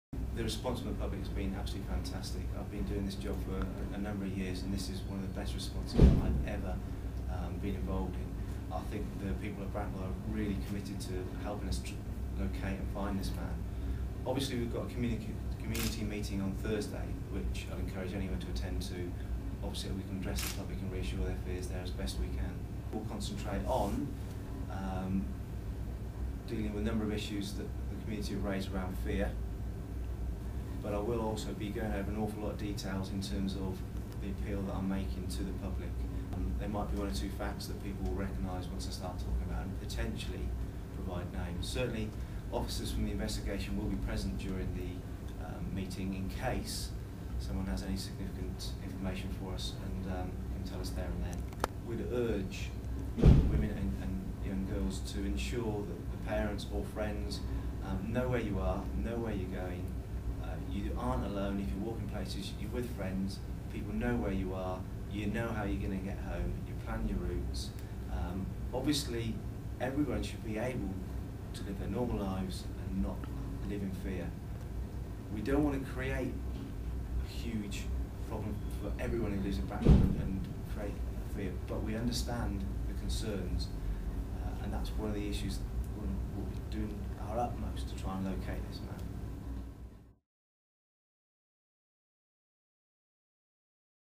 Public meeting in sex attacker hunt
Detective Superintendent Rob Mason urges concerned people to attend a public meeting organised by officers hunting a serial sex attacker in Bracknell. He also offers advice to women and girls.